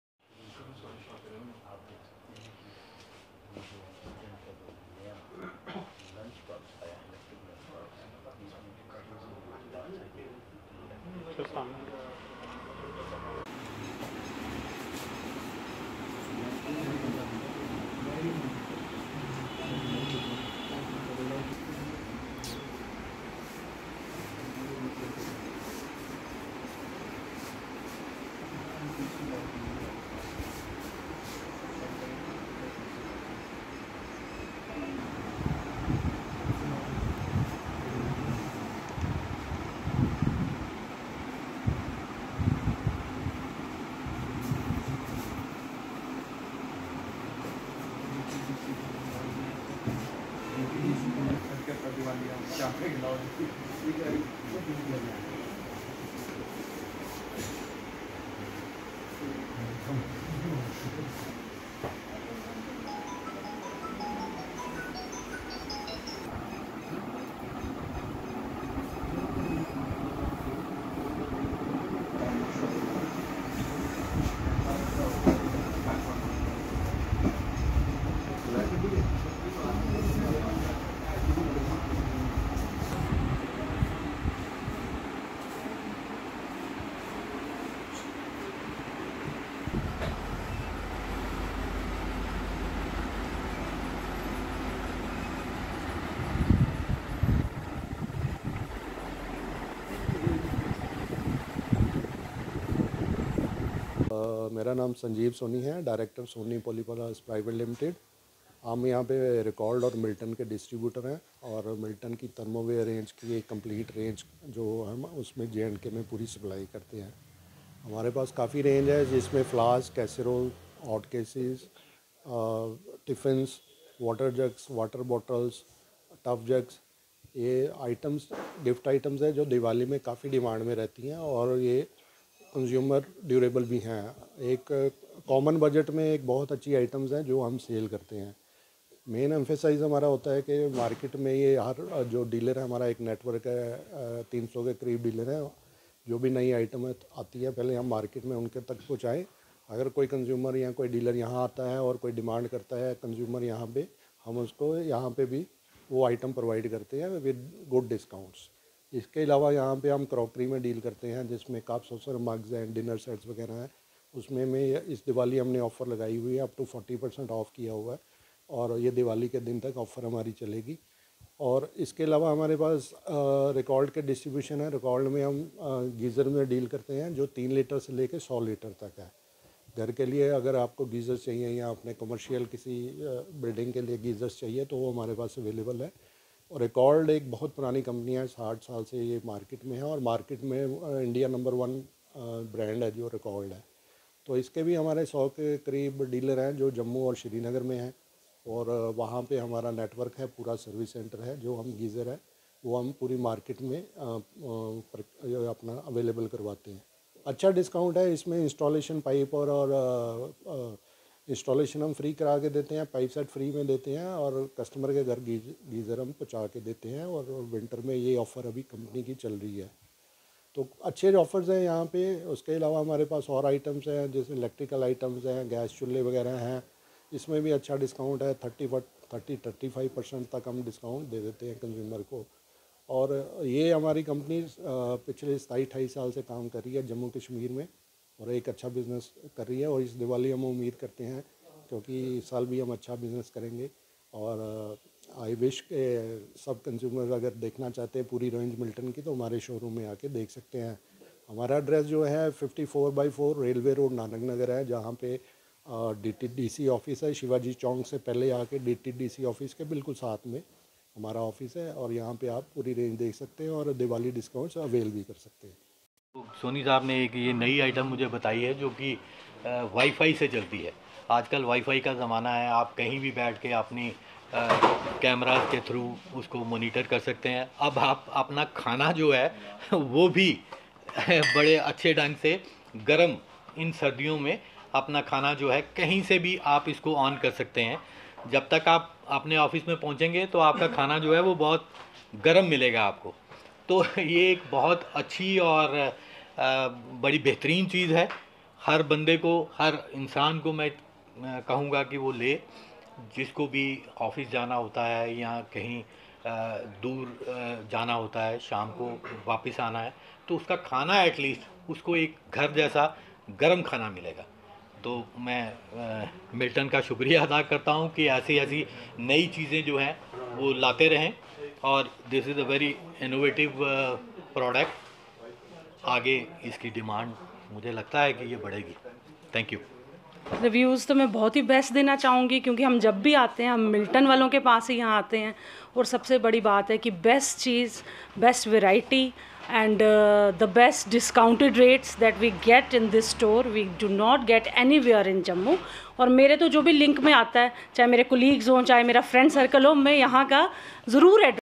Soni Polyplas Pvt Ltd is a 27 years old Company doing distribution business
Soni Polyplas Pvt Ltd is a 27 years old Company doing distribution business. They are distributor of Milton Thermoware and Racold Distributon. The Company has its head office at 54/4, Railway Road, Nanak Nagar, Near DTDC office. Company has its branches at JDA Complex, City Chowk Jammu as well as at Nowgam Chowk, Srinagar

Sanjeev Soni, Managing Director of the Company is looking after day to day business of the Company. Sangeeta Soni, Director of the Company looks after marketing and sales. Company has a large dealer network of more than 200 hundred dealers in through out the state.

This Diwali Milton has brought many new gift items for Consumers. Milton has a vast range of Flasks, Caserolles, Hot cases, Electric Kattle, Electric Tiffins, Water Jugs, Bottles and Tuf jugs. The Company is offering very good discount on bulk purchases. On casserole set Company has Discount offer upto 35%.

Other than that Soni Polyplas is also dealing with Crockery items with a good discount on Mugs, Cup and Soucers, Dinner sets, Tea set.

Company is also distributor for Racold Geysers and have its range from 3 It to 100 It model. The Company is offering free installation and pipe set on purchase of Racold Geysers.

Sanjeev Soni